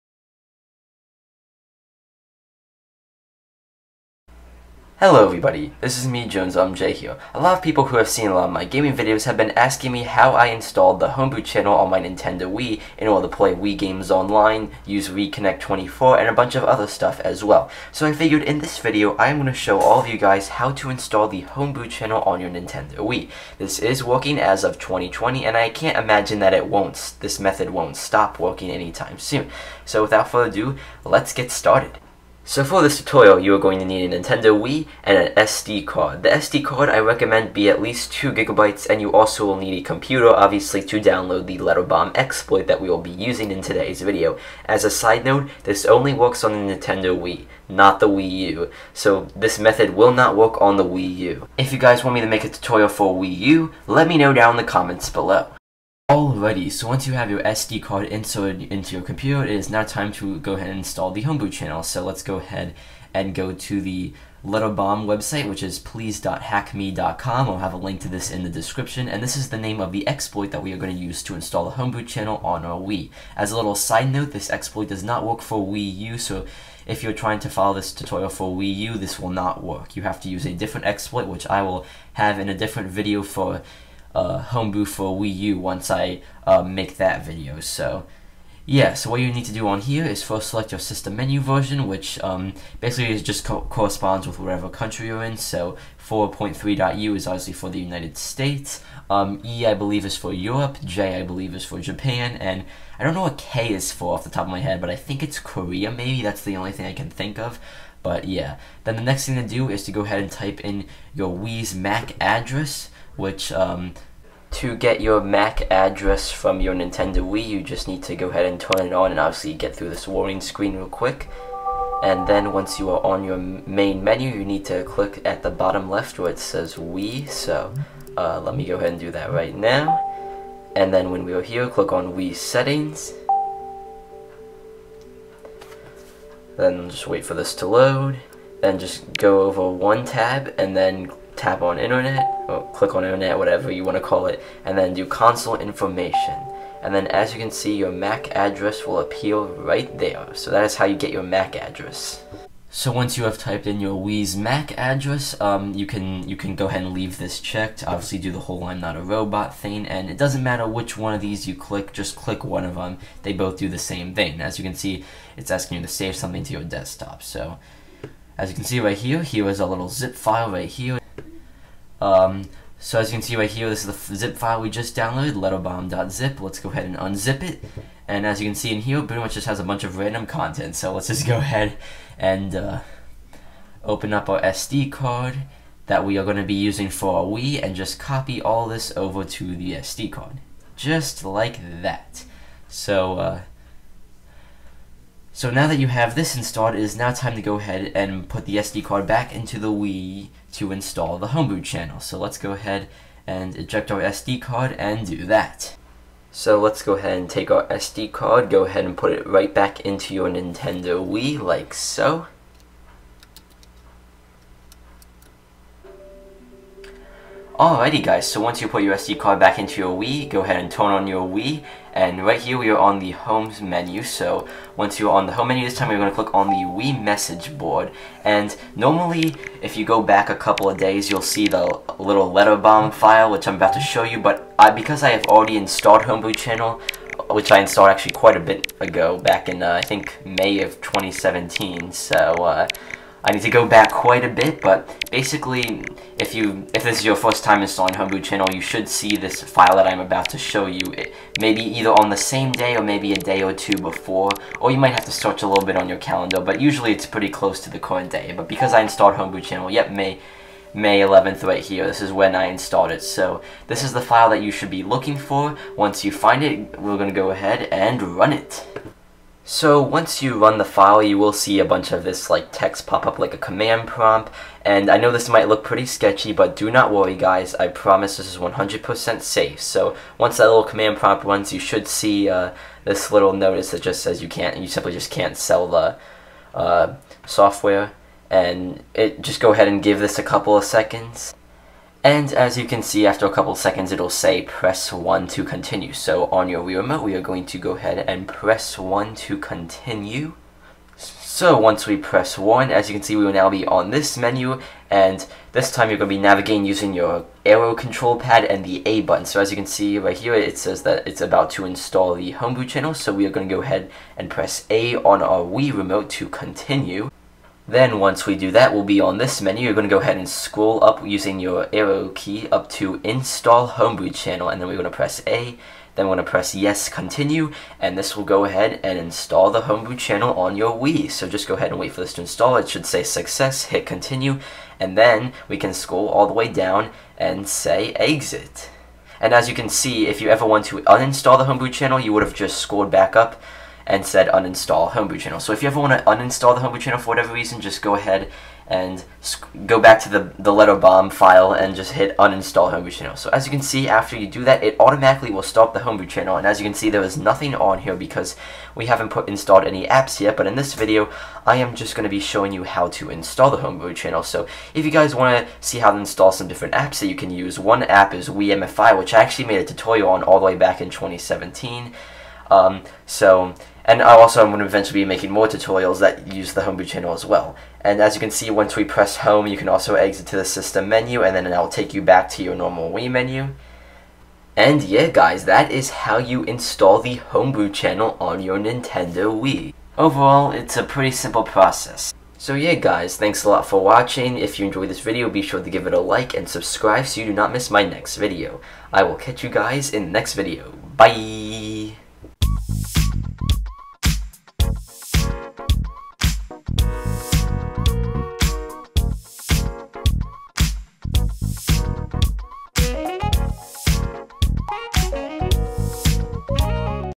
How to Install the Homebrew Channel on the Nintendo Wii (2020)
The Homebrew Channel allows you to run apps on the Nintendo Wii not officially licensed by Nintendo. It also lets you play wii games online with wiimmfi and can be used to bring back WiiConnect24 with RiiConnect24! This tutorial will walk you through the installation process for the Homebrew Channel on the Nintendo Wii! I hope you find this helpful! Enjoy! :)

Timestamps
0:00 Introduction
0:57 Go to the Letterbomb Exploit website
1:51 How to find your Wii's MAC Address
2:33 Downloading the Letterbomb Exploit
2:53 Formatting the SD Card
3:37 Running the Letterbomb Exploit
6:00 Conclusion
6:25 Outro

Links from Video:
Prefer a written tutorial?